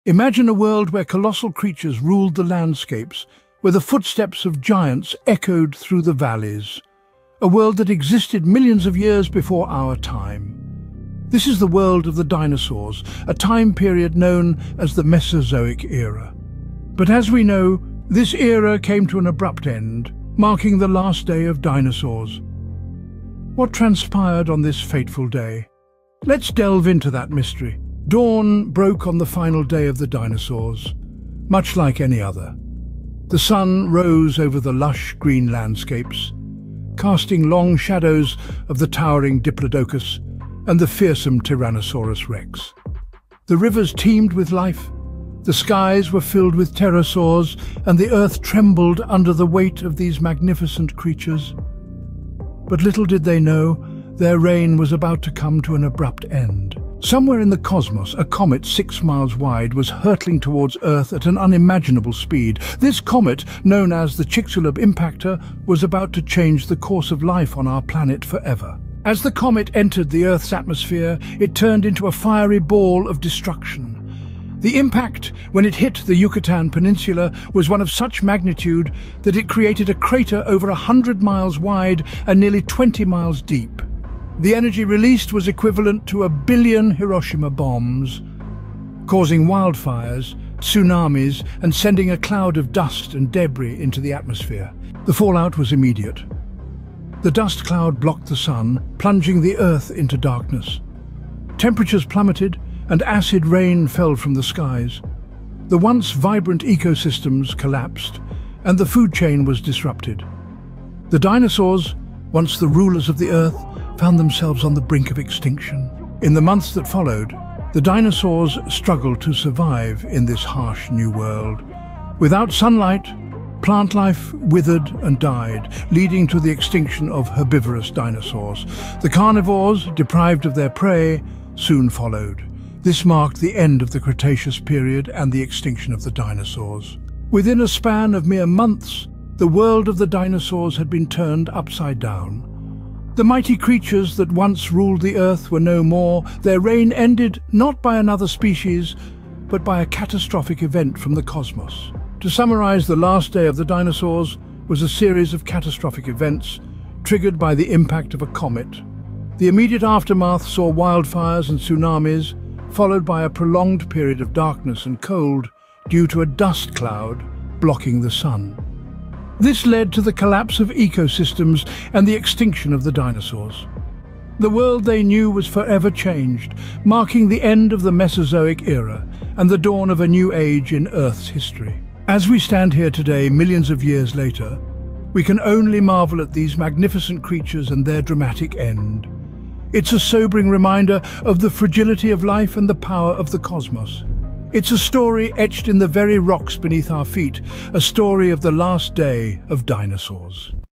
The Final Roar: Last Day of Dinosaurs
OUTLINE:
00:00:00 The World of the Dinosaurs
00:00:28 Dawn of the Final Day
00:00:56 The Comet Approaches
00:01:08 The Impact
00:01:34 The Fallout
00:01:56 Struggling to Survive
00:02:17 The End of an Era
00:02:32 The Last Day of Dinosaurs
00:03:02 Reflections on the End